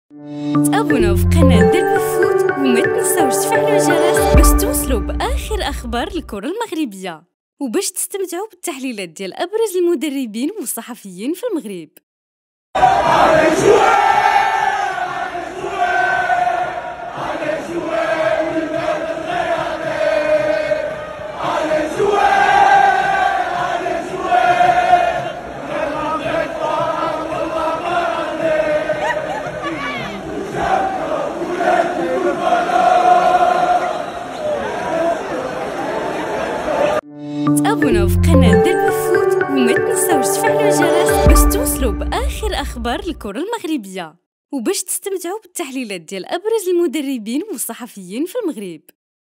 ما لم تشاهده على الرياضية : كراكاج خيالي للوينرز يولع مدرجات دونور
🙏🙏 المرجوا دعمنا من خلال الإشتراك بالقناة و تفعيل الجرس، 🚨 كما لا تنسوا 👍 وضع الإعجاب للفيديو و تعليق 🙏🙏